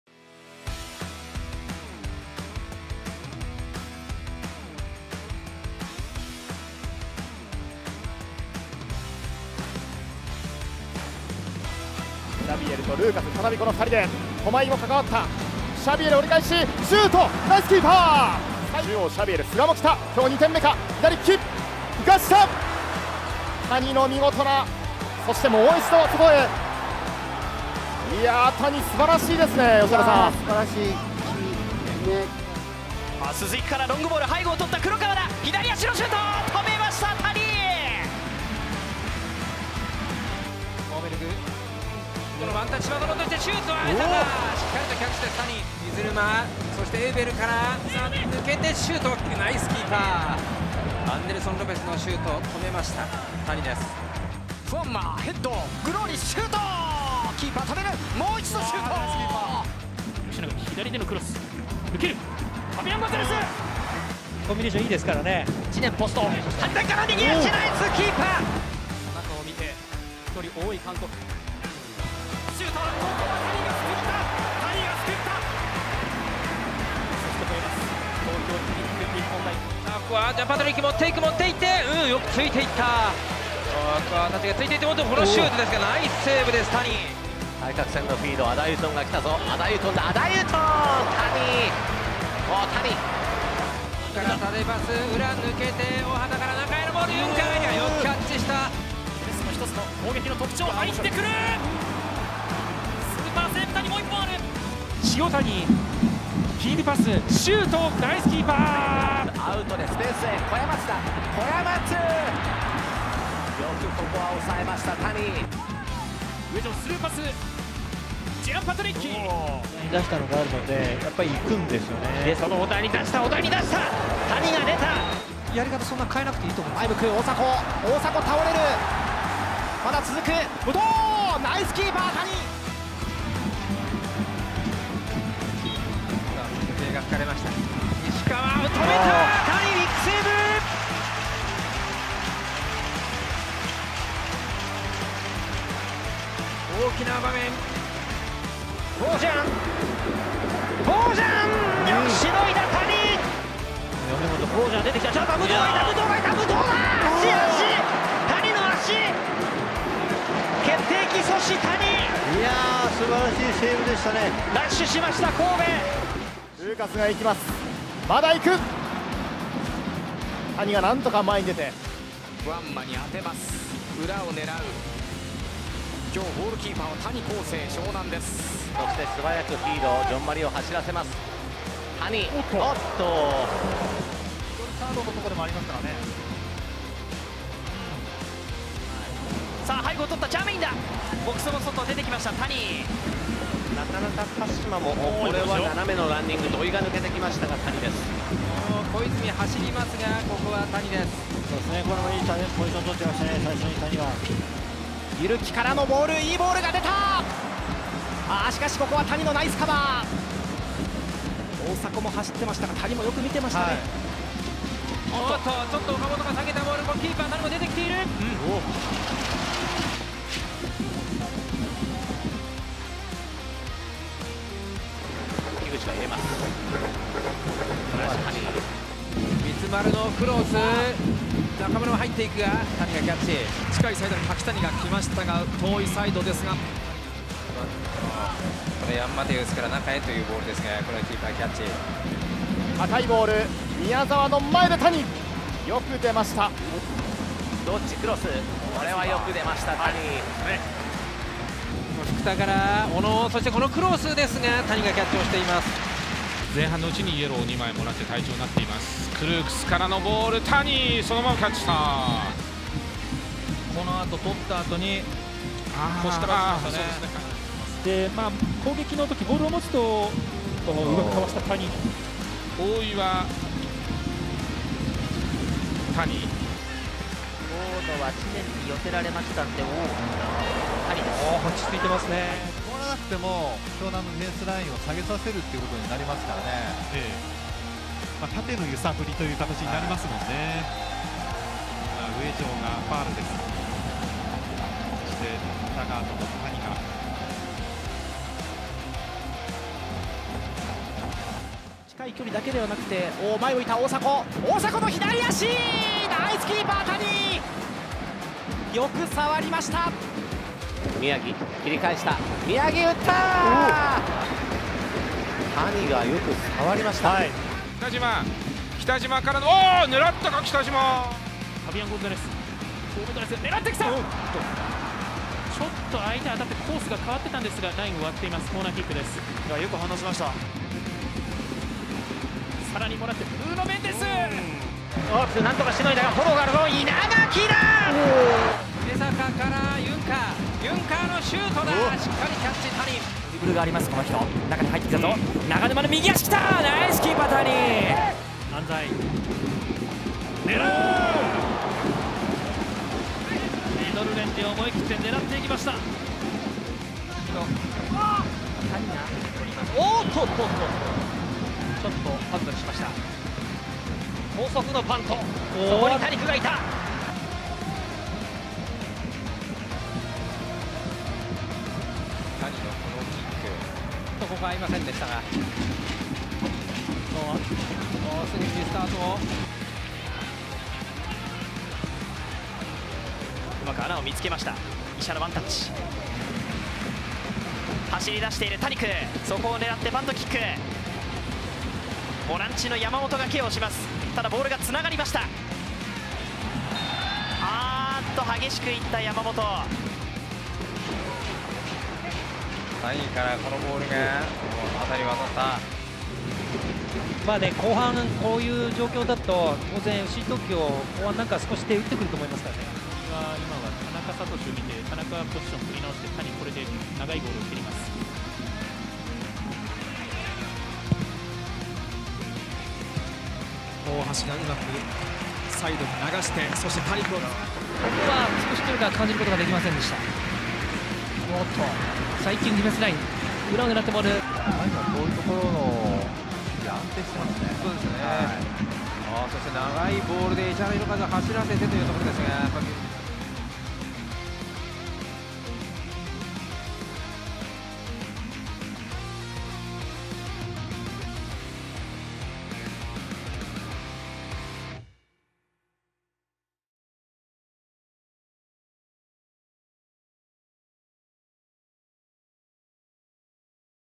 谷晃生の湘南修行 2022🔥
0:00 イントロ
0:12 エリア内シュート等のセーブ
2:47 エリア外への飛び出し
3:43 ハイボールキャッチ
4:27 足元のスキル
5:02 エリア外シュート等のセーブ
5:57 キック

【湘南ベルマーレ/GK1/谷 晃生】
2000年11月22日生まれ/22歳/190cm/84kg/大阪府/利き足:右
G大阪ジュニアユース→G大阪ユース→G大阪
・好きなアーティスト:BLACKPINK
・好きな女性芸能人:芳根京子
・よく見るYoutube動画:かまいたちチャンネル